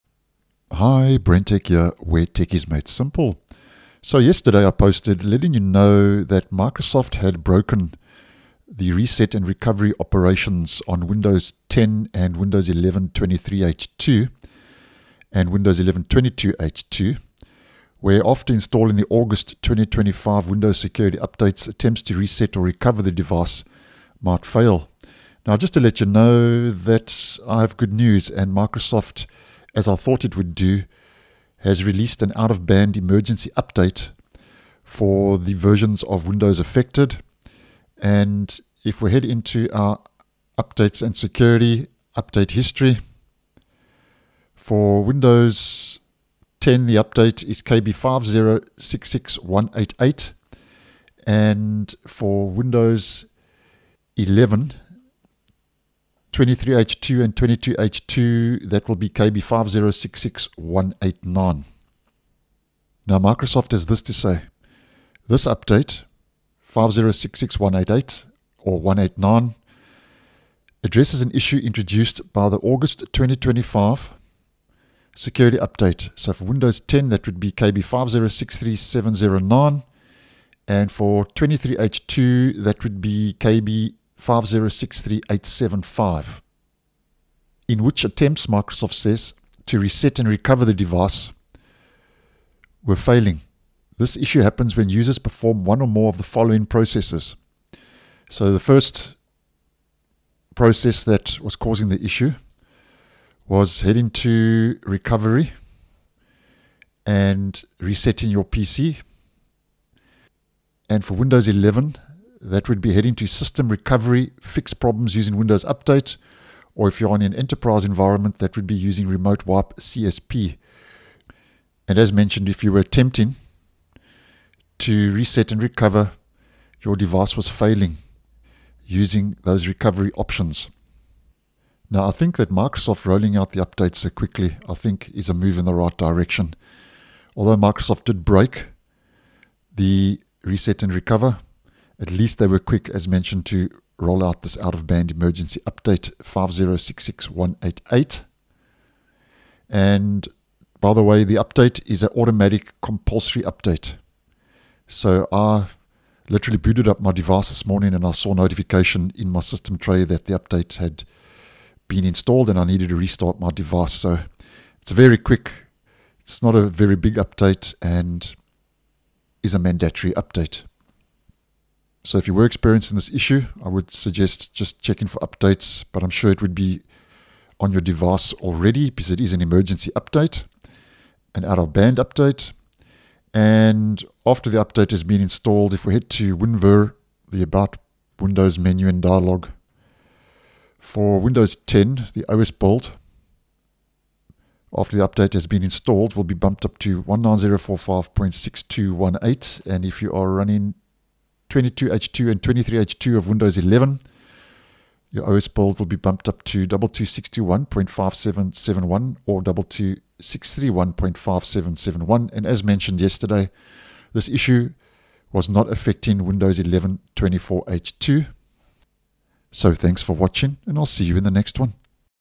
Microsoft Releases EMERGENCY Updates to Fix Broken Windows 10 & 11 Recovery! (KB5066188 & KB5066189)
These Out-of-band updates address an issue introduced by the August 2025 security updates.